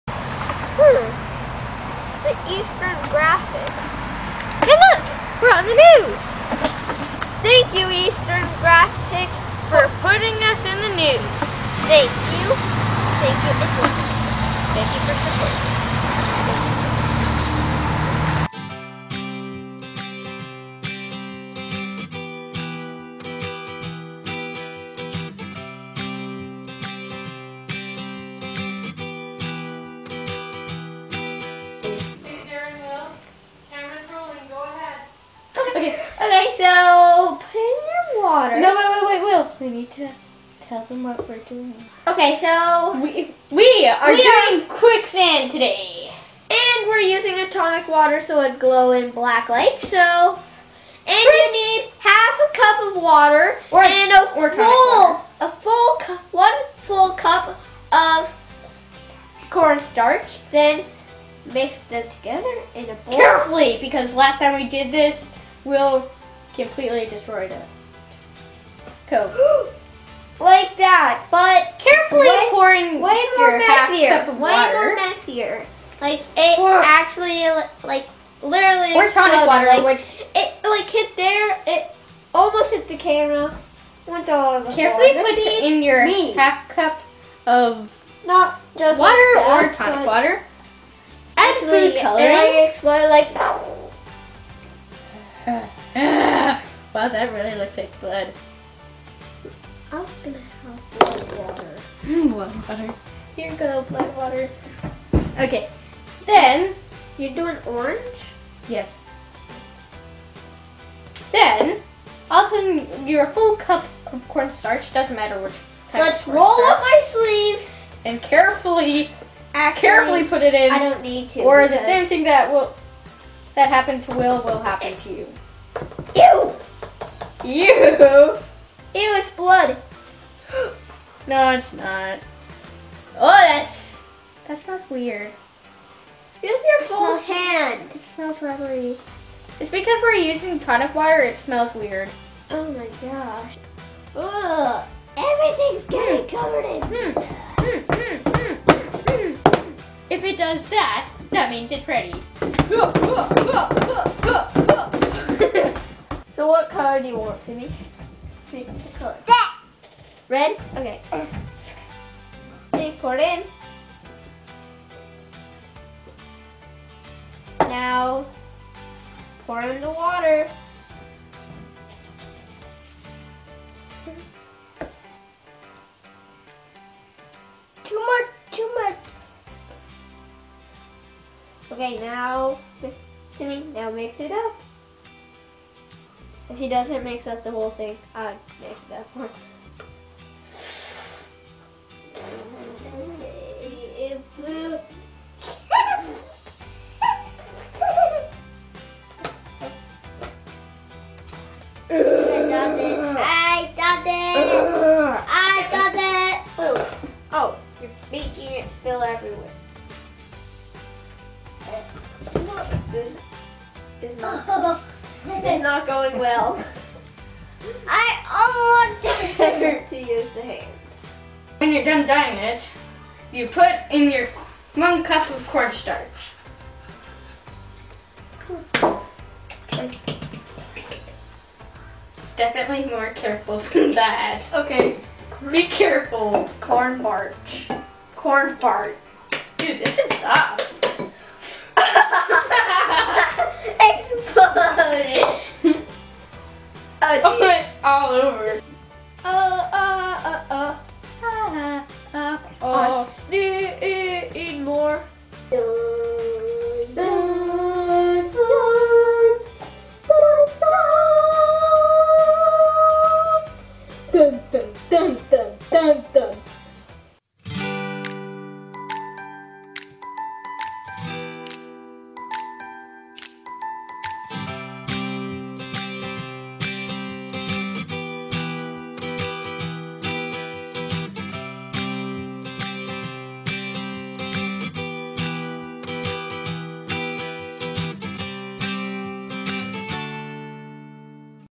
Quicksand - The Science Dragons
WARNING: Do not dump in the sink as it will clog.

Sarah, Will and Sam make Quicksand!  Mix a half cup corn starch with 1 cup water.  Add more corn starch or water as needed to get the desired honey-like consistency.  Be careful, this is really messy, but once it dries, it is super easy to clean up.